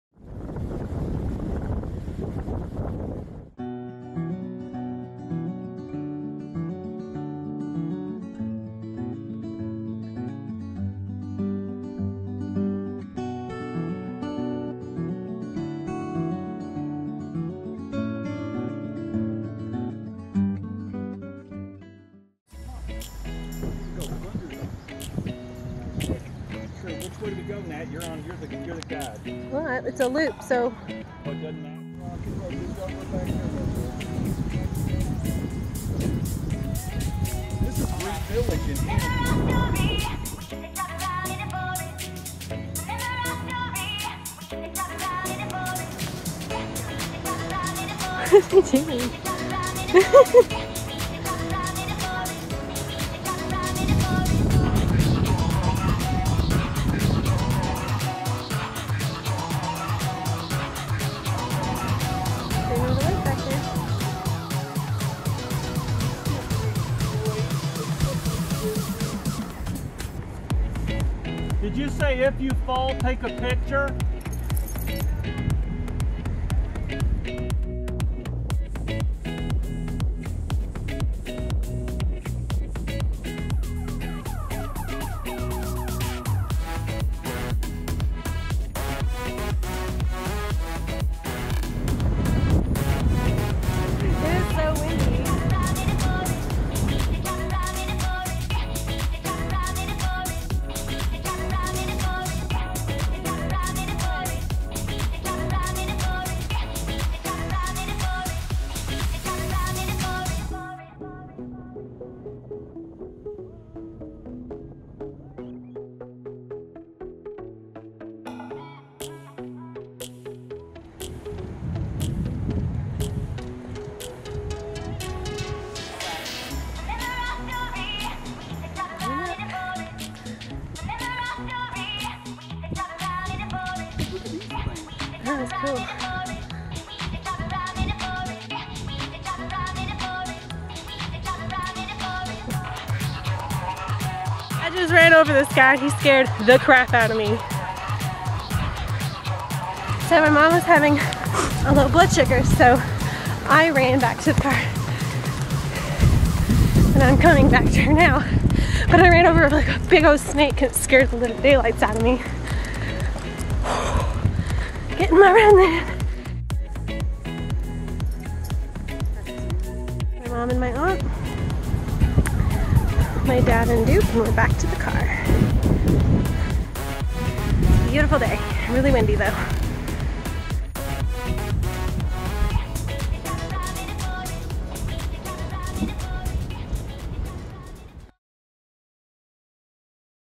Green River Lake Hiking | The Day I Ran Over A SNAKE! | Vlog #20
Day hiking Green River Lake!

Thank you for checking out my video! My family and I went on a hike around Green River Lake. It was a lot of fun because it was one of the first 80-degree days this Spring and the wind was strong! The lake had decent waves!

-Nat